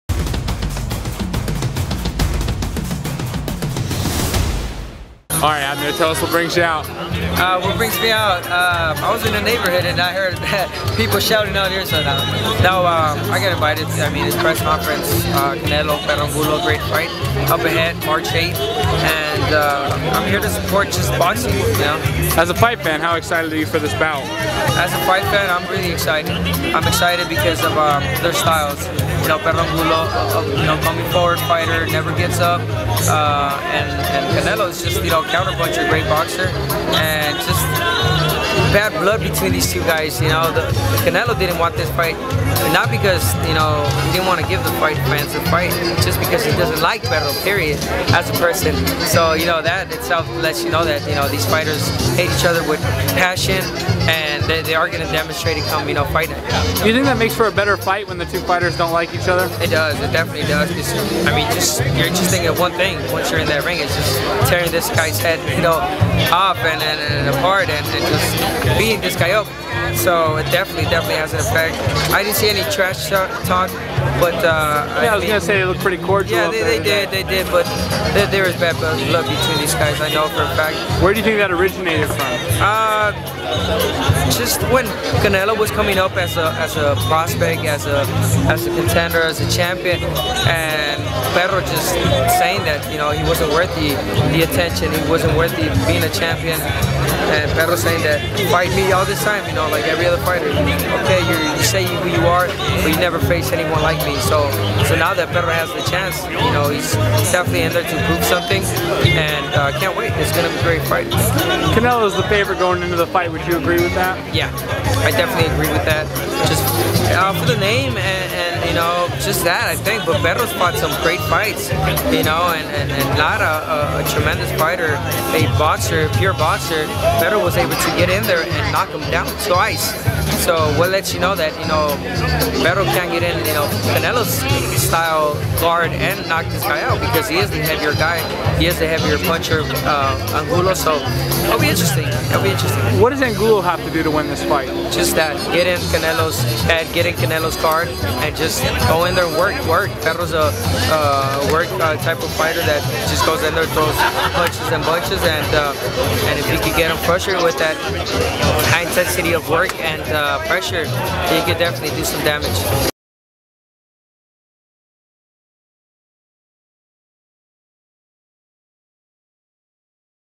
Abner Mares "Canelo & Angulo hate each other"
Layzie The Savage speaks to Abner Mares about Canelo Alvarez vs. Alfredo Angulo and reveals there is legit bad blood between the two.